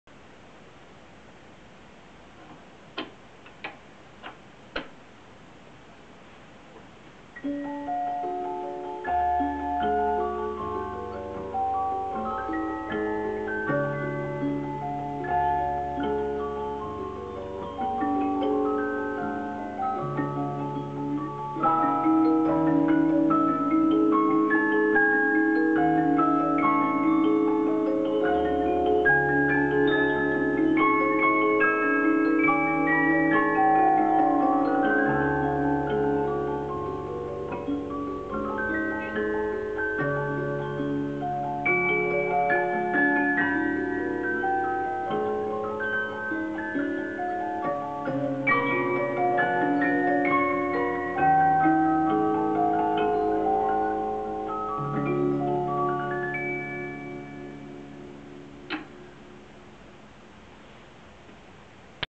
19th Century Music Box (Lot 186, Sale F070320)
A 19th century Swiss music box, playing 8 airs, possibly Nicole Freres, the song card to inside lid 'Fabrique de Geneve', the movement stamped '1644' '45', cylinder inscribed '1644' and 'G416', some pencil marks to base, in very good fully restored condition, 63 by 24 by 20.5cm high.